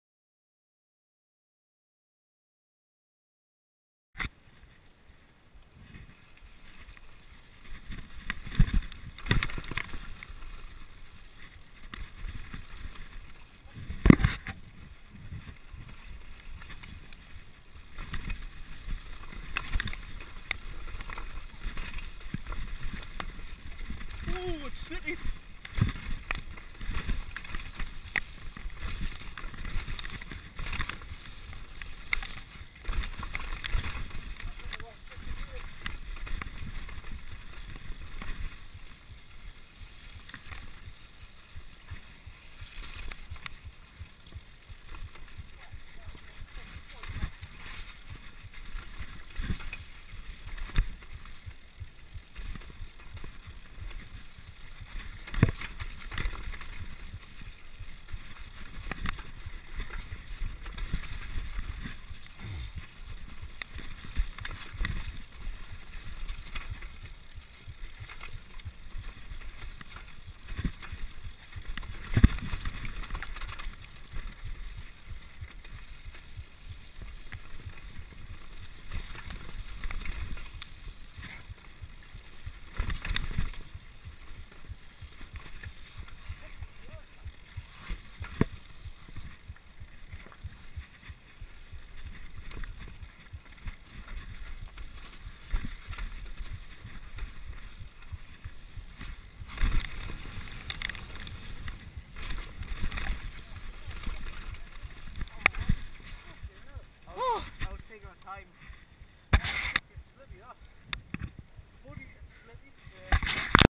Hamsterley forest Downhill tracks
This is me, kev and lewis doing the downhill tracks at hamsterley forest.